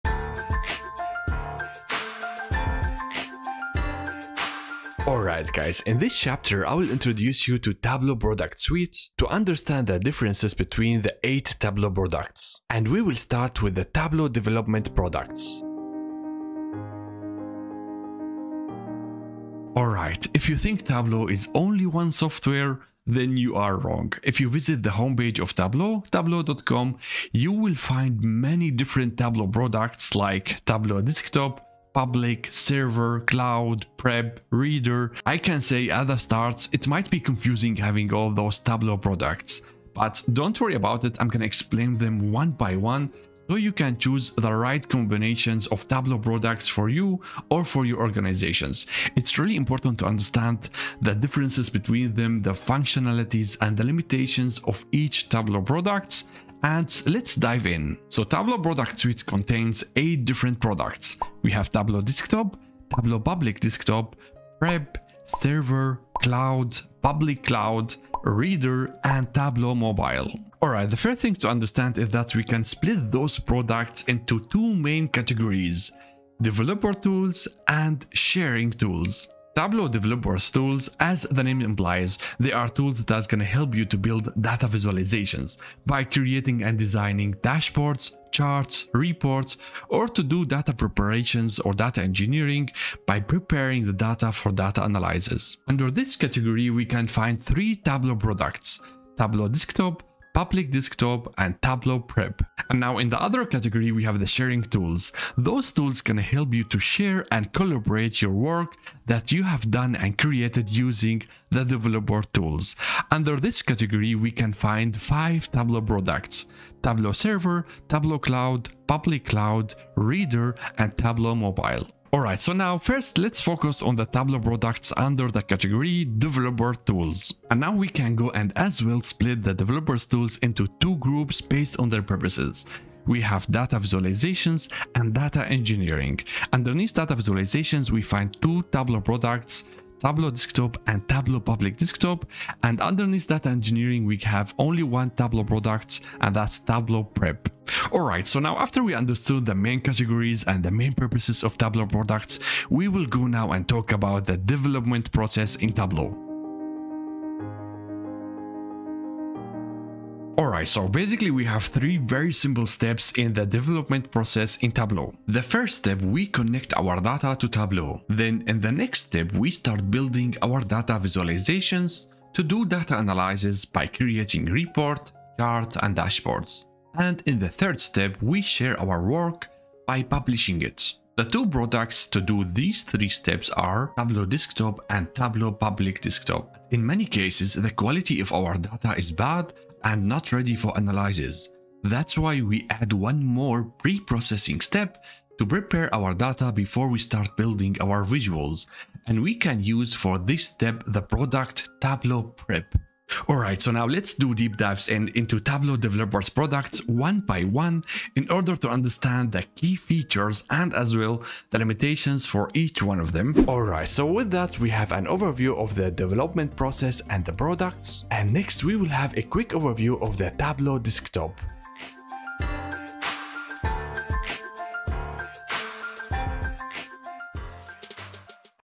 Tableau Development Products & Process | Tableau Course #8
In this video, I'll show you an overview of Tableau's product offerings.

*MY COURSES*
To get certified, better clean structure & progress, support me: